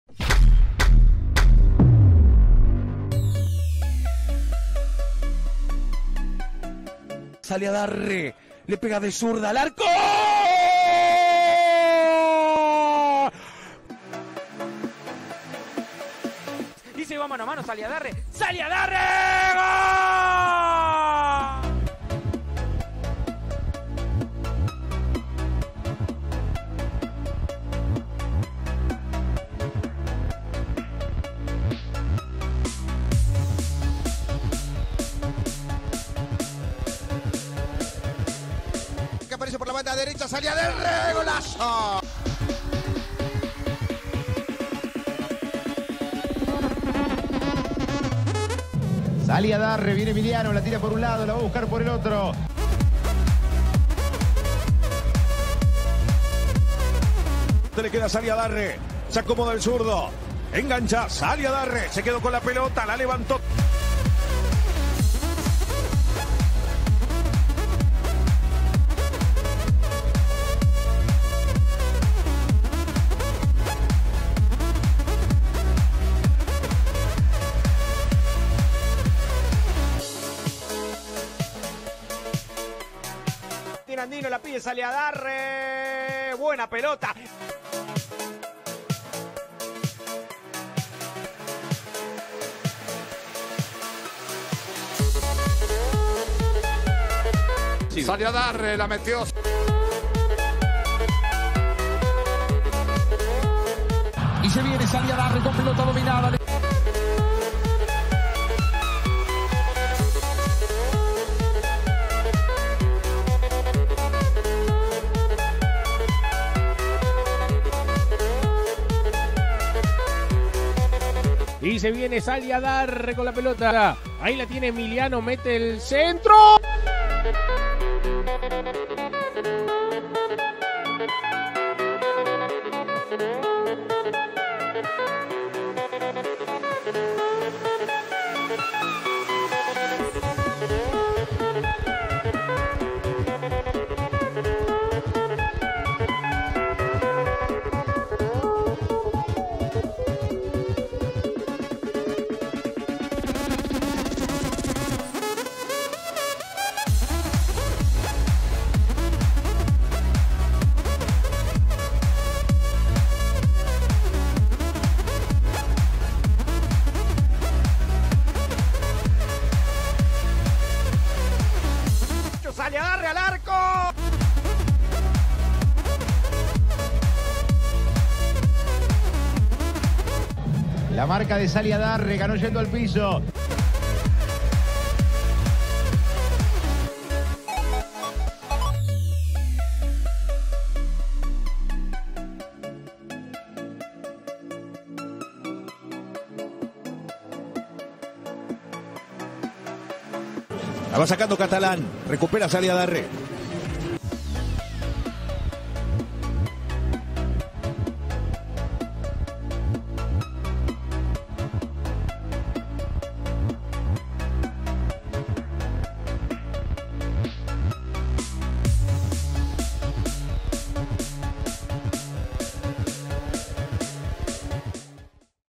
Emiliano Saliadarre - Avios Soccer
Compacto de jugadas de Emiliano Saliadarre, delantero de Temperley (Argentina).